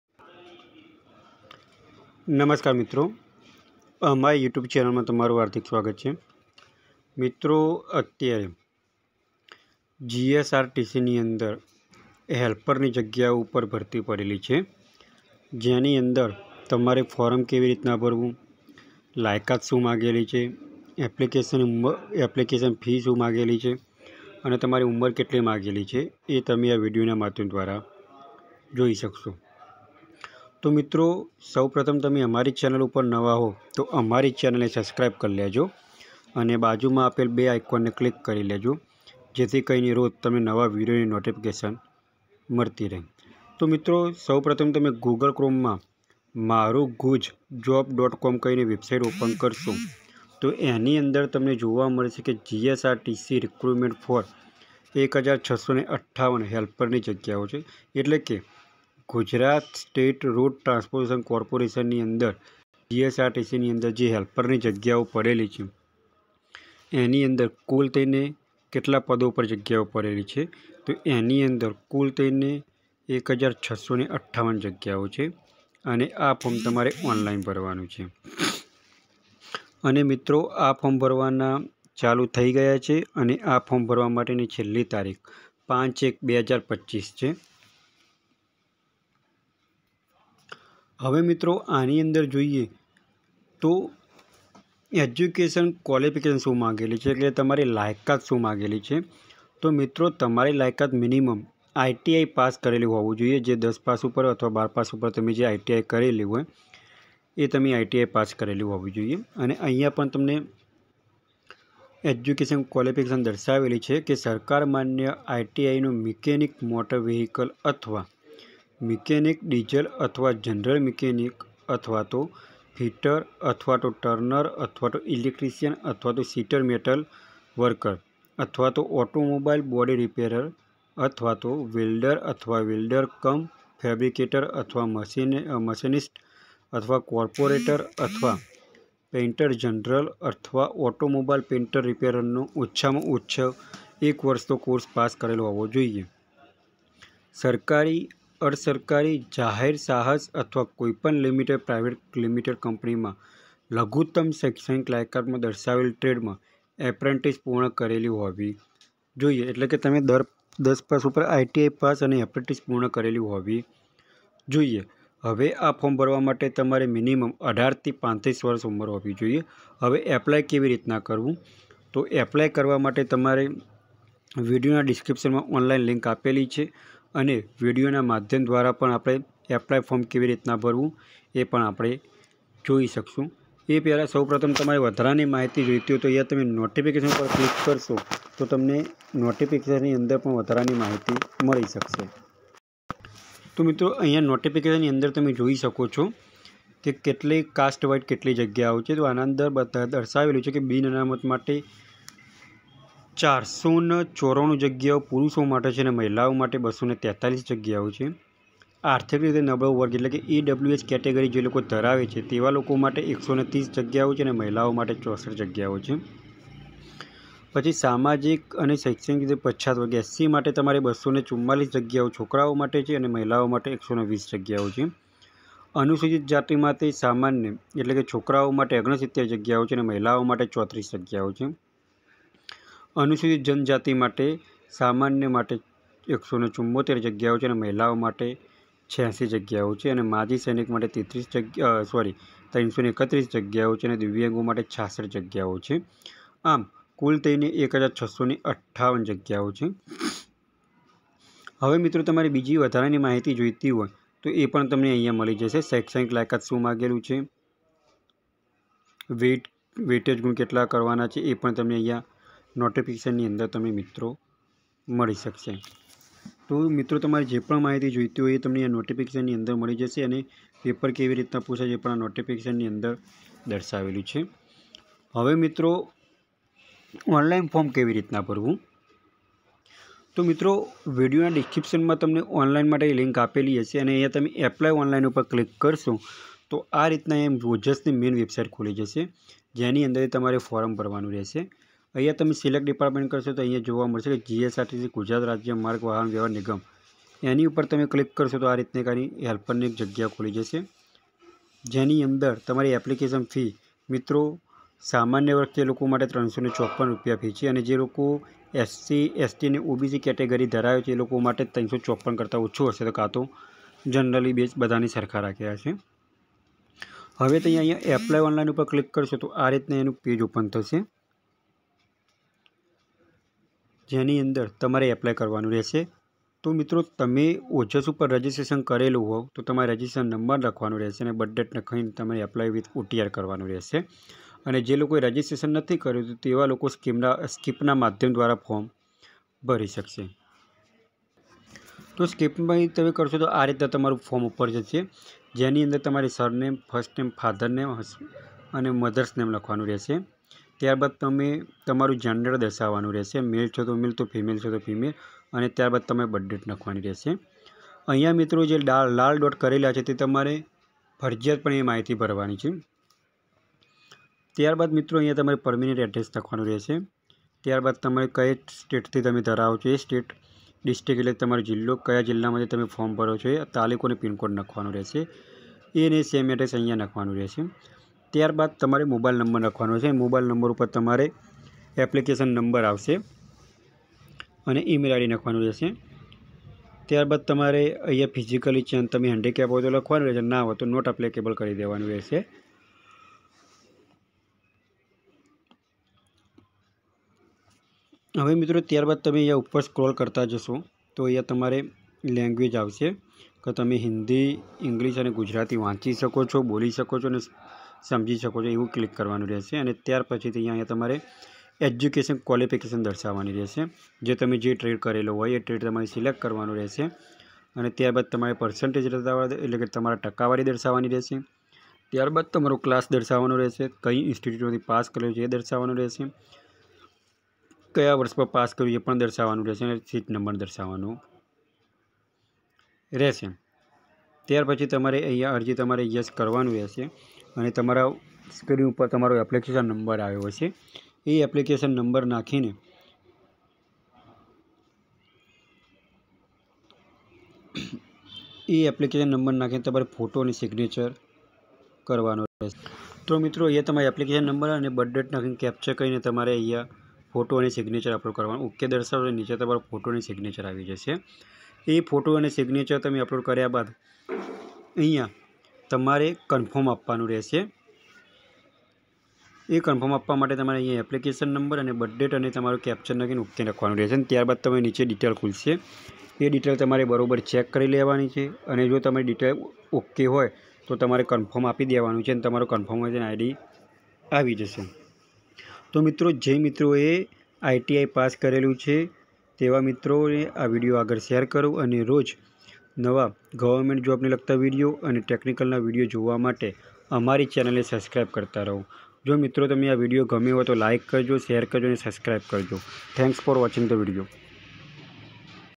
GSRTC Recruitment for 1658 Helper Posts 2024
Gujarat State Road Transport Corporation (GSRTC) has published advertisement for various posts. Within which the eligibility, age limit, fee, last date and how to fill the form for different posts are published in this website. Eligible candidates are advised to fill this recruitment. Keep checking the  website marugujjob.com daily to get the latest updates.